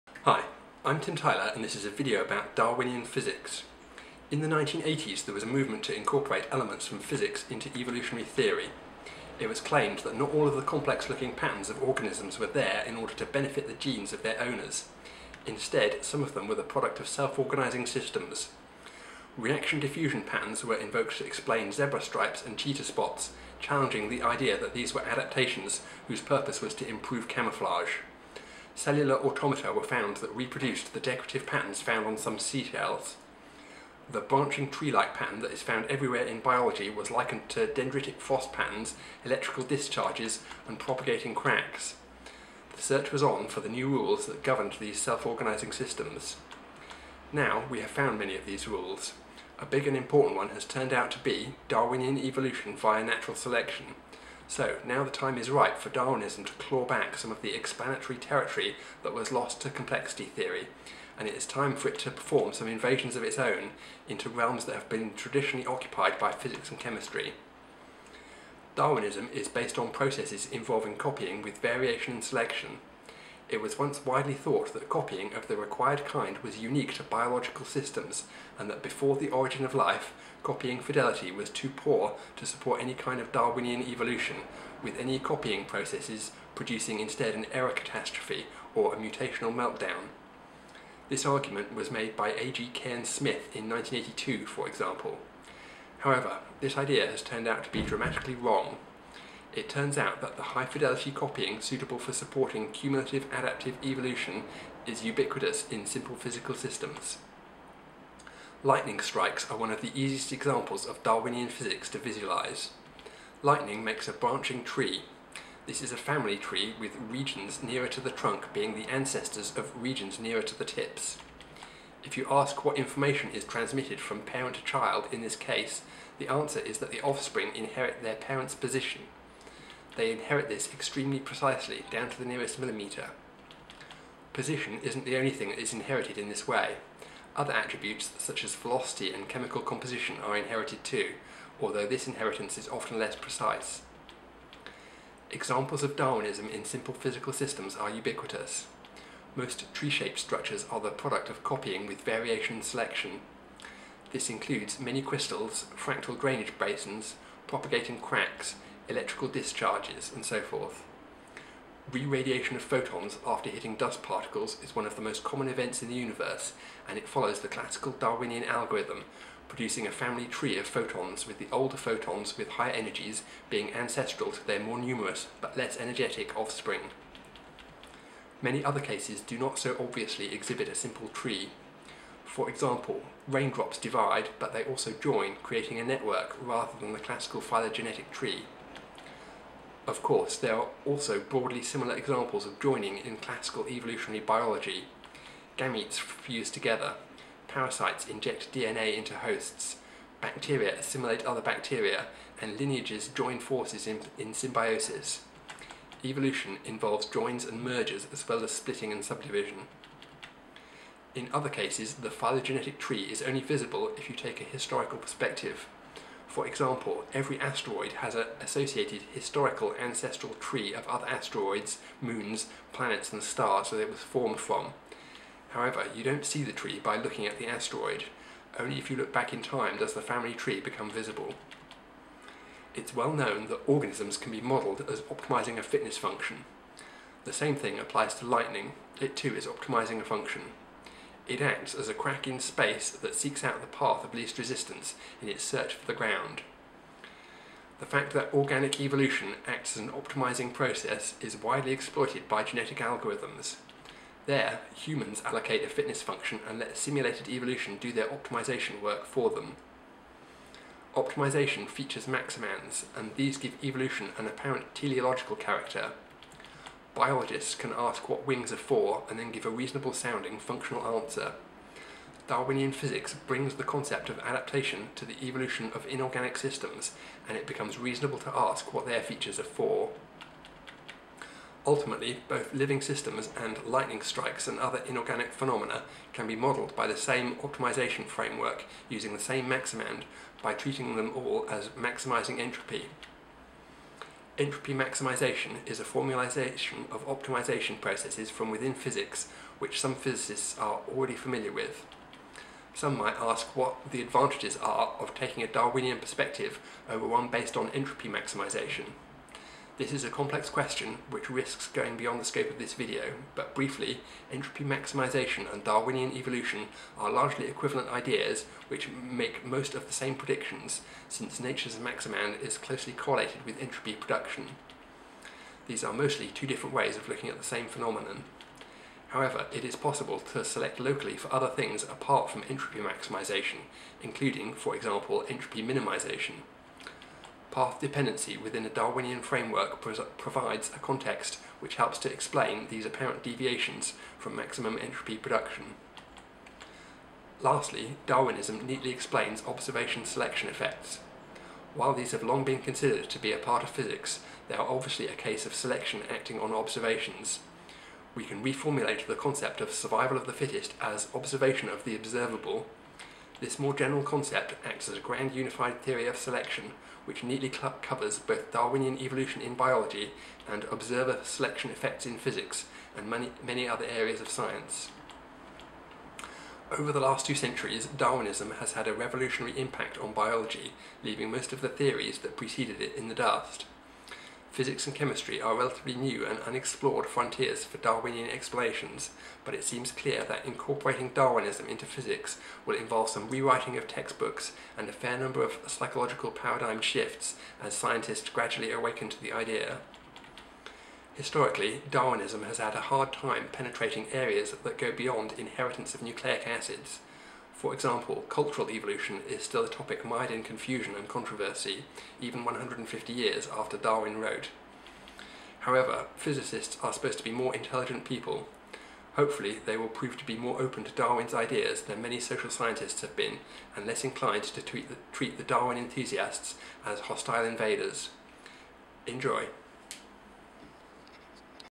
Tim Tyler: Darwinian physics
Now the time is ripe for Darwinism to claw back some of the explanatory territory that was lost to complexity theory - and it is time for it to perform some invasions of its own - into realms that have been traditionally occupied by physics and chemistry.